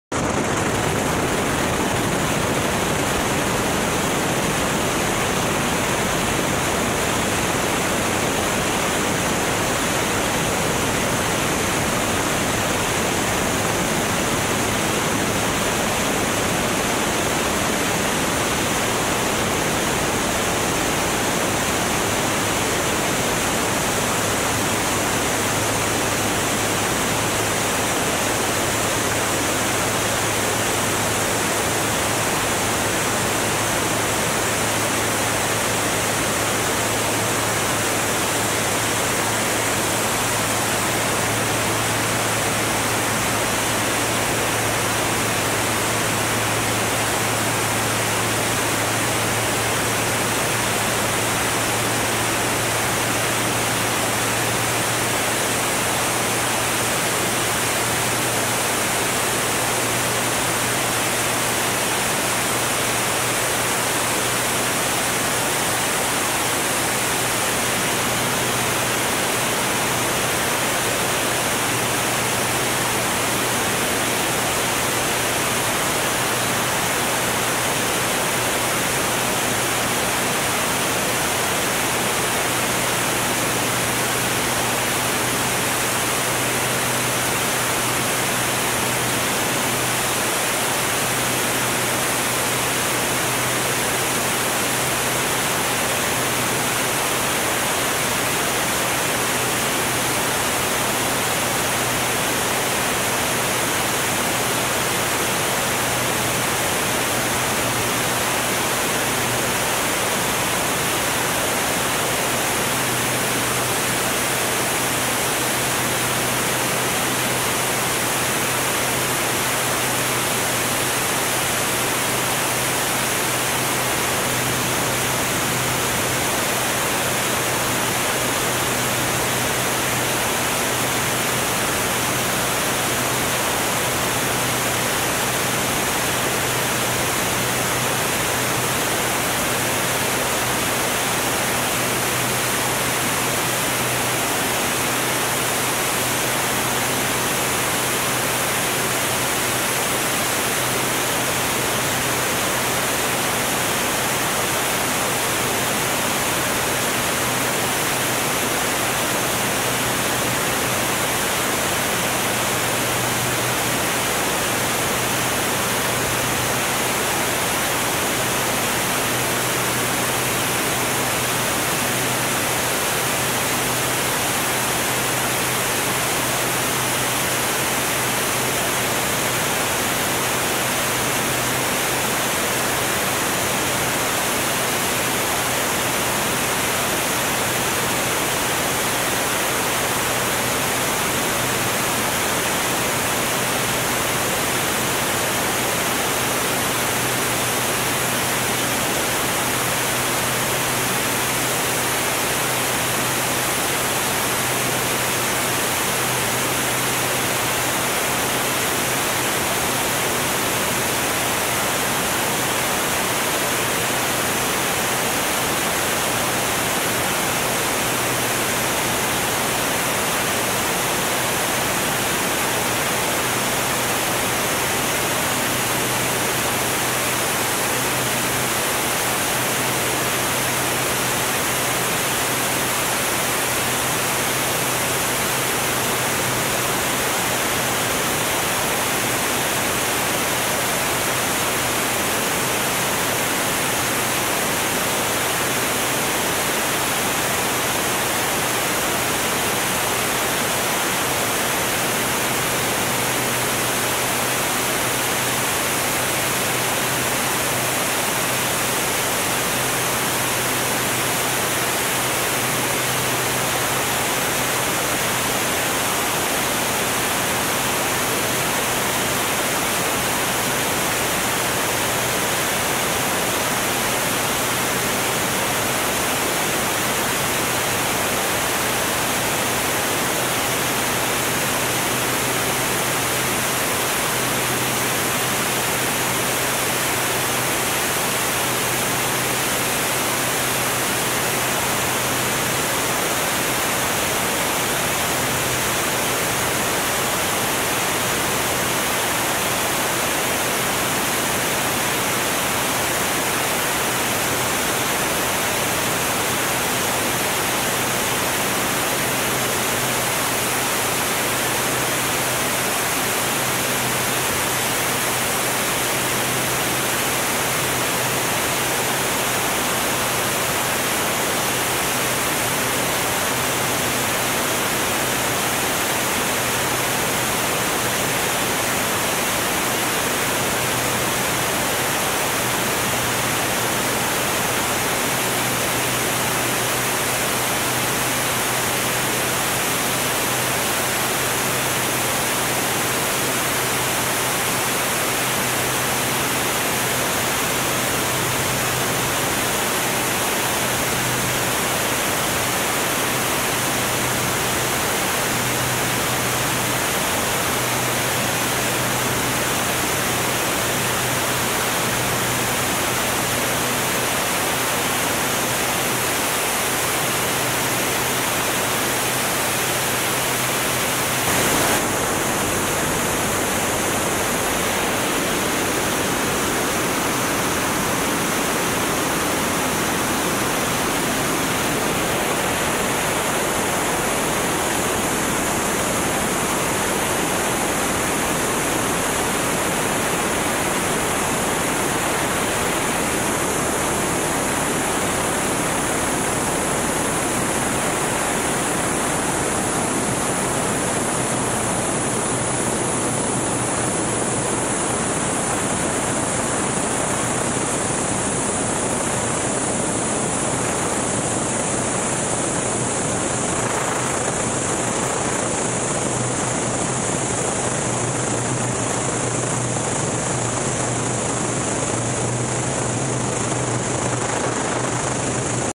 Reno Air Races Formula 1 2019
Wednesday Free Practice- Race 81 & 79 Formation